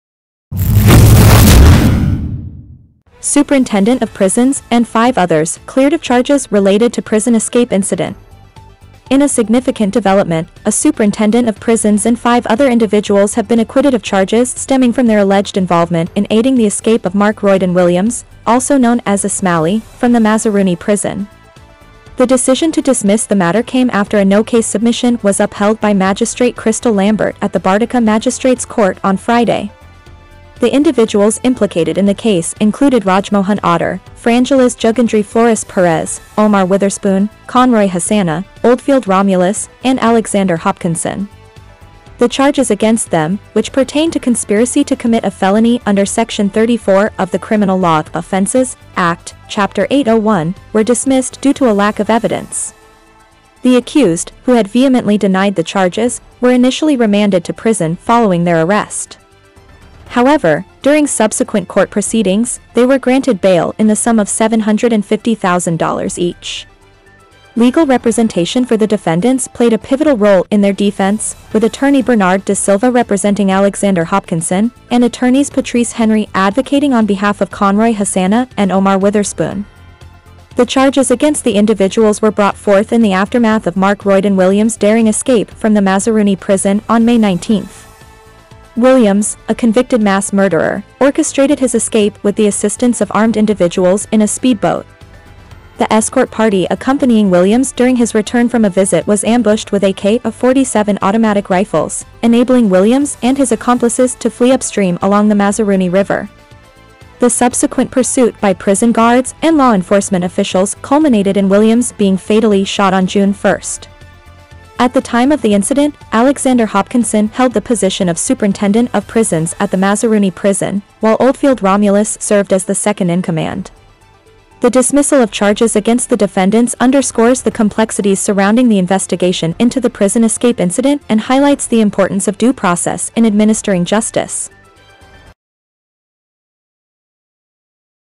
Superintendent of Prisons and Five Others Cleared of Charges Related to Prison Escape Incident
In a significant development,  a Superintendent of Prisons and five other individuals have been acquitted of charges stemming from their alleged involvement in aiding the escape of Mark Royden Williams,  also known as 'Smallie, ' from the Mazaruni Prison.  The decision to dismiss the matter came after a no-case submission was upheld by Magistrate Crystal Lambert at the Bartica Magistrate's Court on Friday.

The individuals implicated in the case included Rajmohan Autor,  Frangeliz Jugandry Flores Perez,  Omar Witherspoon,  Conroy Hosannah,  Oldfield Romulus,  and Alexander Hopkinson.  The charges against them,  which pertained to conspiracy to commit a felony under section 34 of the Criminal Law (Offences) Act,  Chapter 8:01, were dismissed due to a lack of evidence.

The accused,  who had vehemently denied the charges,  were initially remanded to prison following their arrest.  However,  during subsequent court proceedings,  they were granted bail in the sum of $750,000 each.

Legal representation for the defendants played a pivotal role in their defense,  with attorney Bernard Da Silva representing Alexander Hopkinson,  and attorneys Patrice Henry advocating on behalf of Conroy Hosannah and Omar Witherspoon.

The charges against the individuals were brought forth in the aftermath of Mark Royden Williams' daring escape from the Mazaruni Prison on May 19. Williams,  a convicted mass murderer,  orchestrated his escape with the assistance of armed individuals in a speedboat.  The escort party accompanying Williams during his return from a visit was ambushed with AK-47 automatic rifles,  enabling Williams and his accomplices to flee upstream along the Mazaruni River.

The subsequent pursuit by prison guards and law enforcement officials culminated in Williams being fatally shot on June 1. At the time of the incident,  Alexander Hopkinson held the position of Superintendent of Prisons at the Mazaruni Prison,  while Oldfield Romulus served as the second-in-command.

The dismissal of charges against the defendants underscores the complexities surrounding the investigation into the prison escape incident and highlights the importance of due process in administering justice.